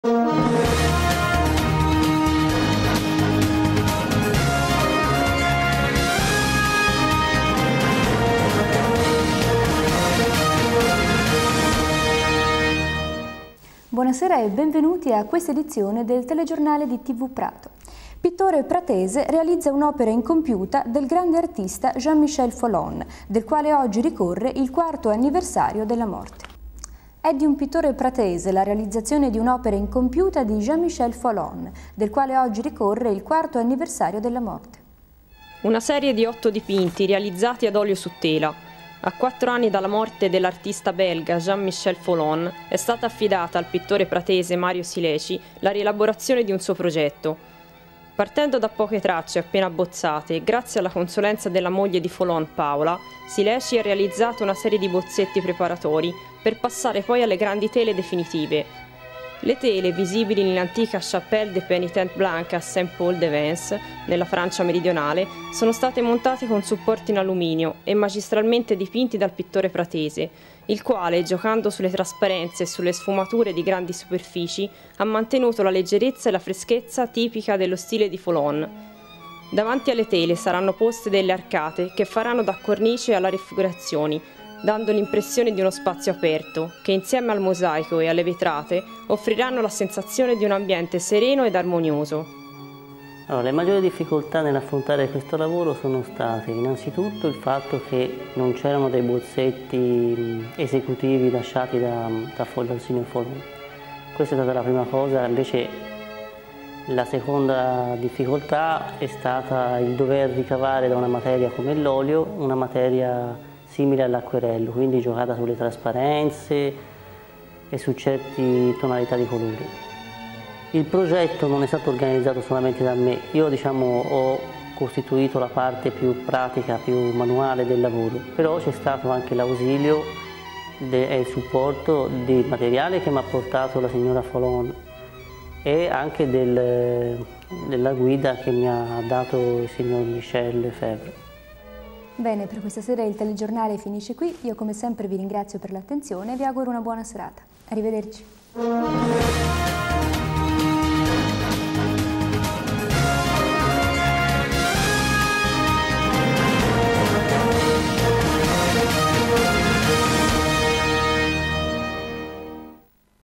Mario Sileci - Folon, progetto incompiuto dell'artista Belga Jean-Michel Folon.
Mario Sileci - pittore Pratese - Prato (PO) Italy - realizza nel 2008 un progetto incompiuto dell'artista Belga Jean-Michel Folon. Il progetto è nella  Chapelle des Pénitents Blancs à Saint-Paul de Vence nella Francia Meridionale.
l’artiste belge Jean-Michel Folon, La Chapelle des Pénitents Blancs à Saint-Paul de Vence (XVIIe-XXIe siècles).

Archivio  di Filippo Sileci.